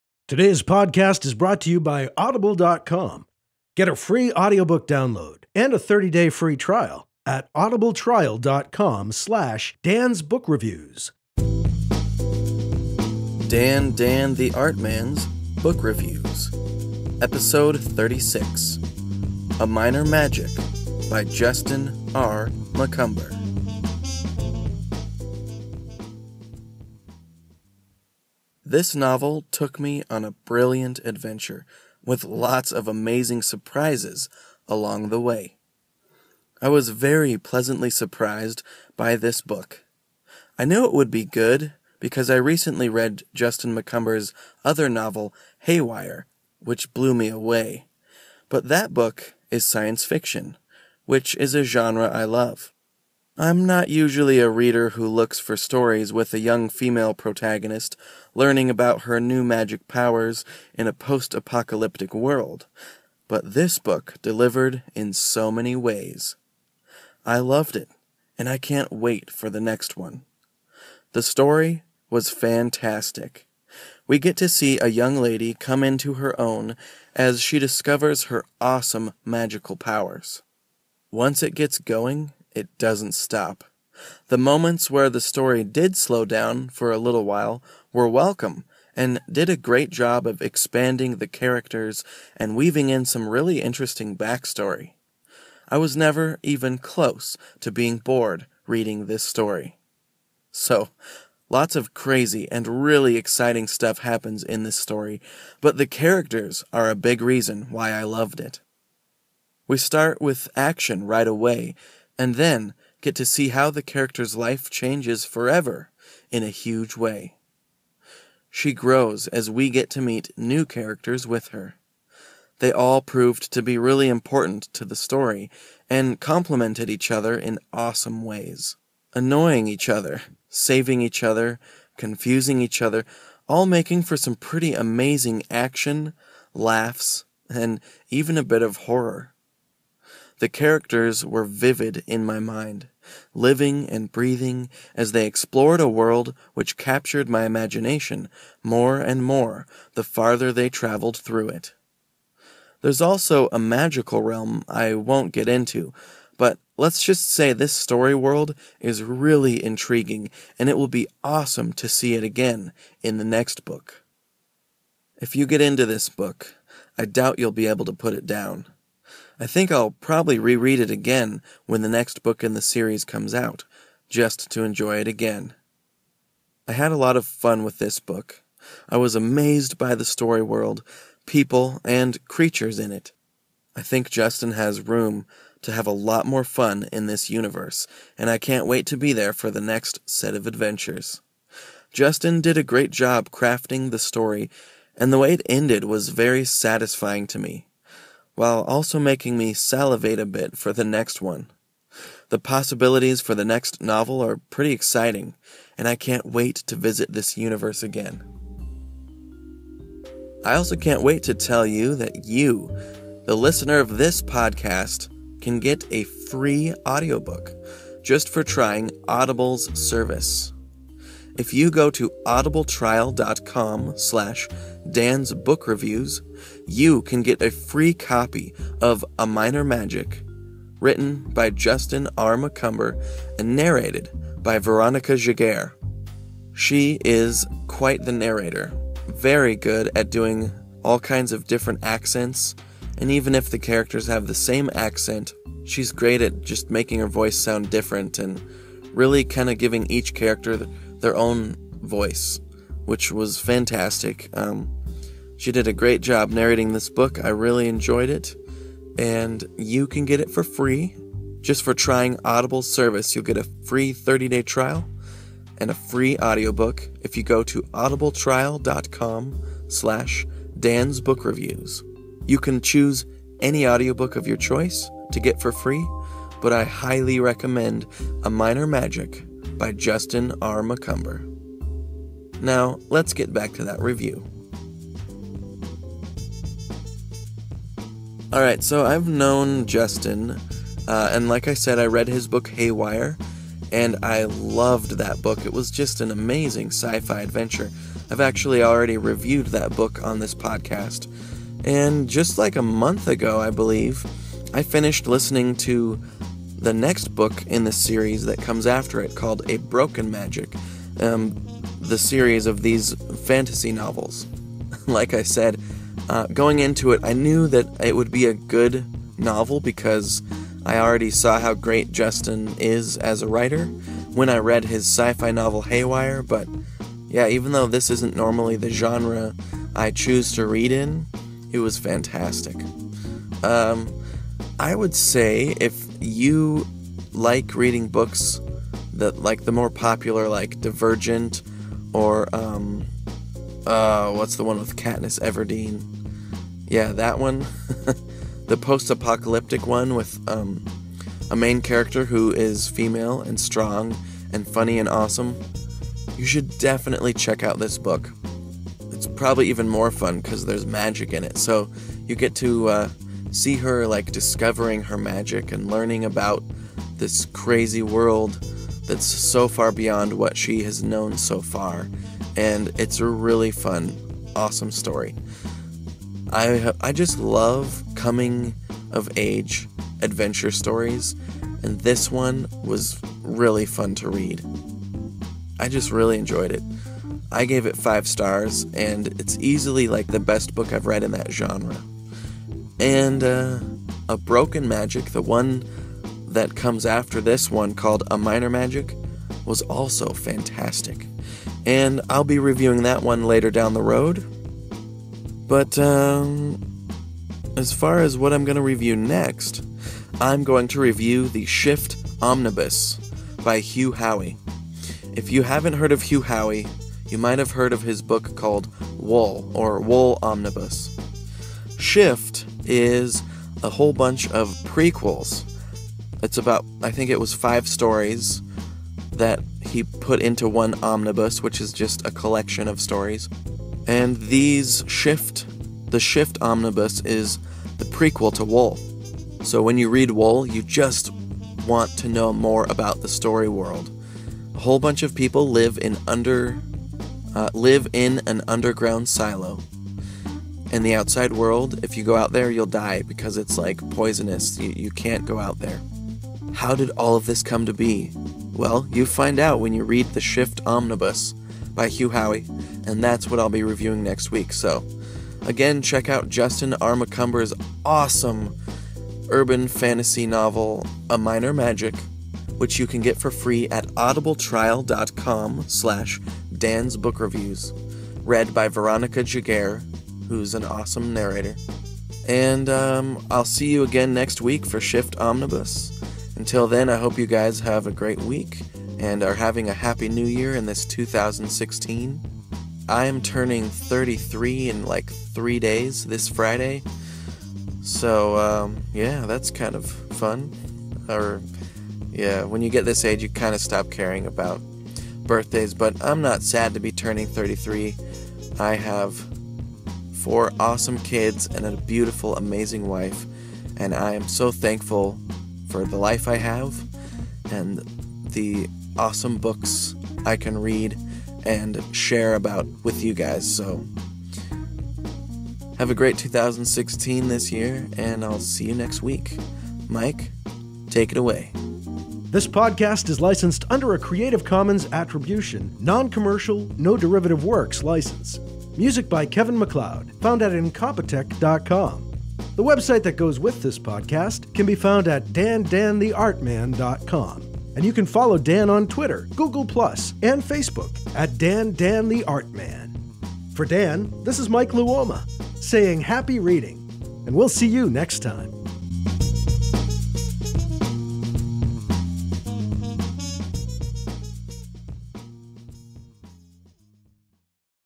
Dan's Book Reviews | Episode 36 - A Minor Magic
In this 36th episode I review the fantastic urban fantasy novel A Minor Magic by Justin R. Macumber. This book is not a genre I usually read but I was very pleasantly surprised at how much I liked it. It was so good! Listen to hear why.